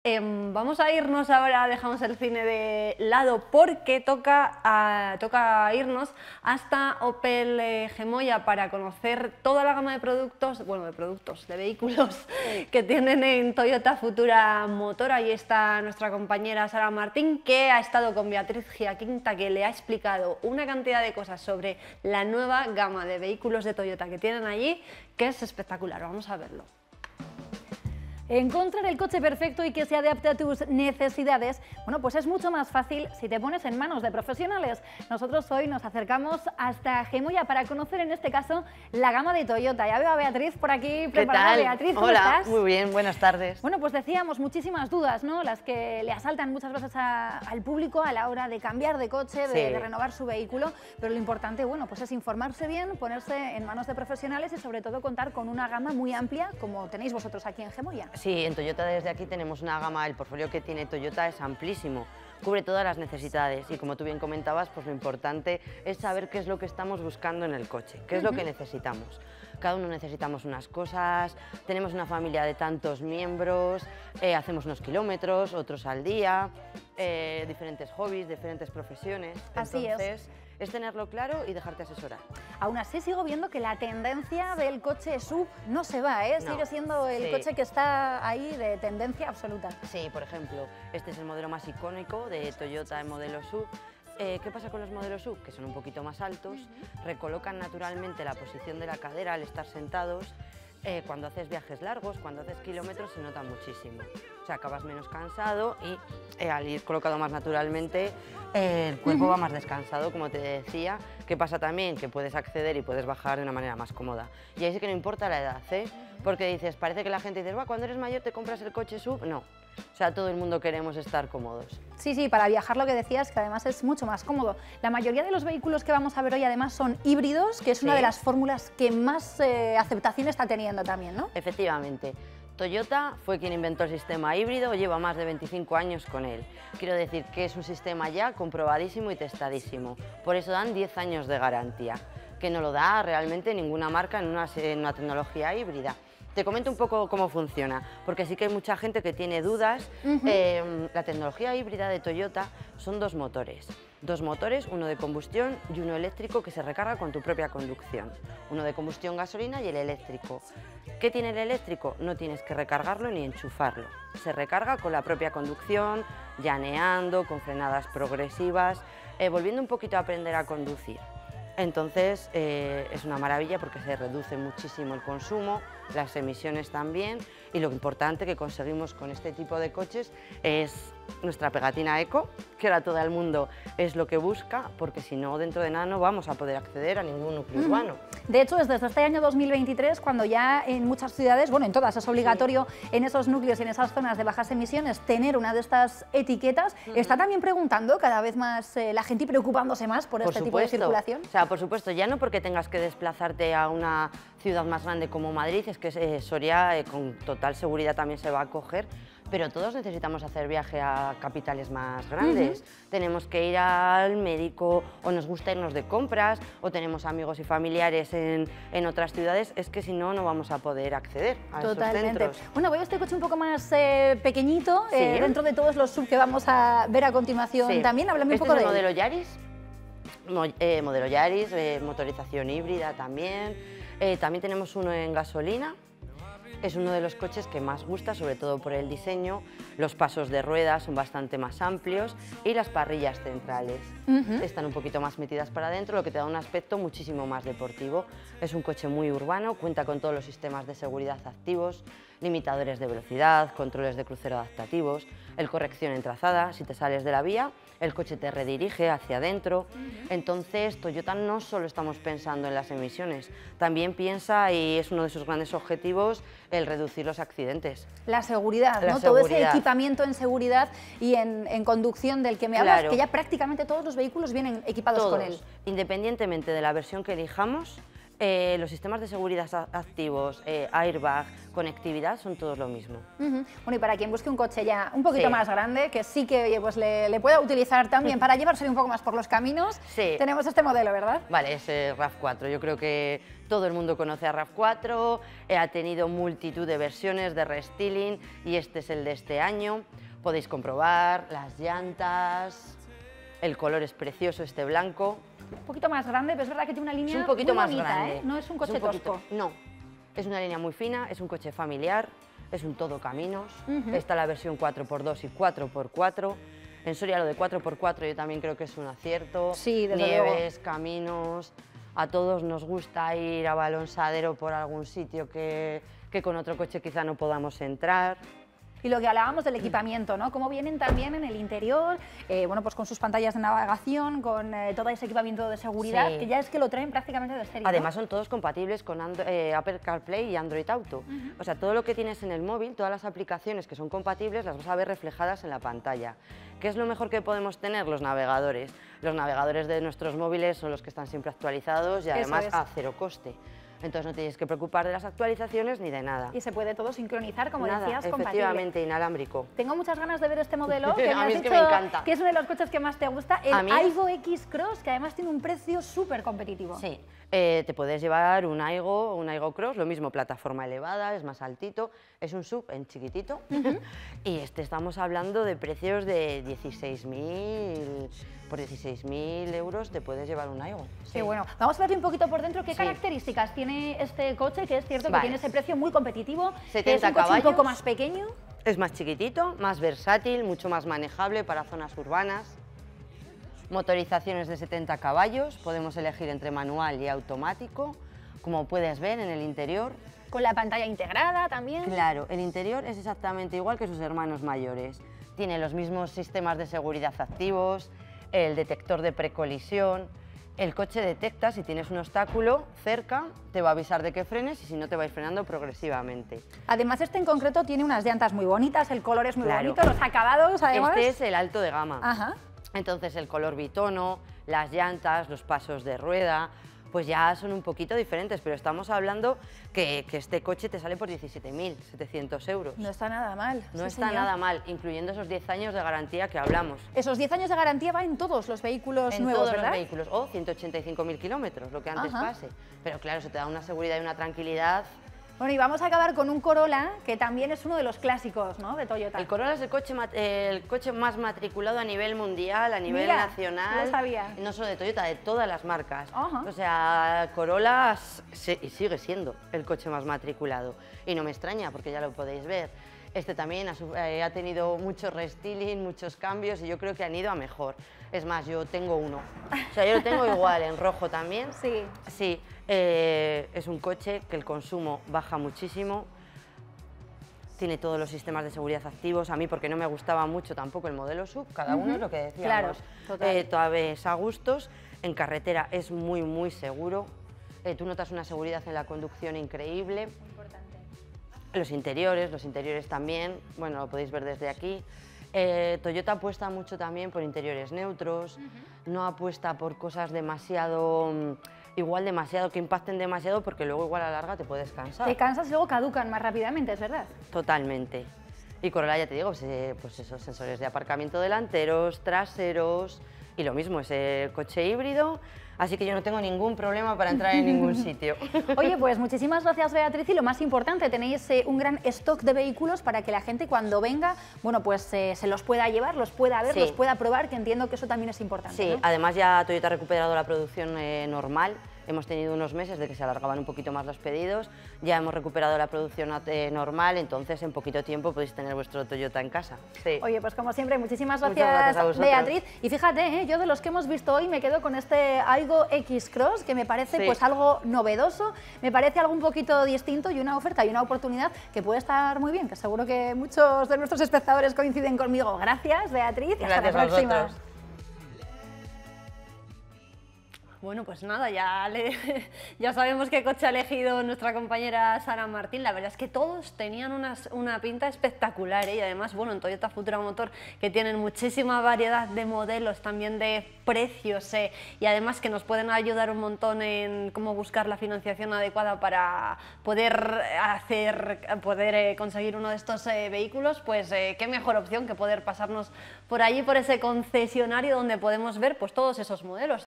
ENCUENTRA EL COCHE DE TU VIDA EN TOYOTA FUTURA MOTOR 14/02/2023
Toyoya es la marca lider en vehículos híbridos, una amplia gama para todas las necesidades, totalmente equipados y con 10 años de garantía.
Desde el Aygo X Cross, Yaris, Corolla, C-HR hasta el Rav 4 todos llevan dos motores , uno de combustión y otro eléctrico que se recarga de manera automática sin necesidad de enchufarlo.